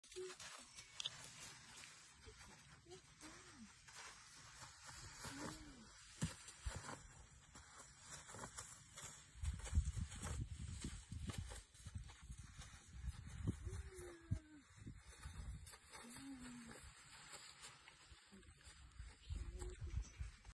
Teaching a yearling to give in to pressure on the stomach
Sideways steps , learning for a yearling to give in to gentle pressure on the stomach . Moving the rear legs Seitwärts ausweichen beim Fohlen.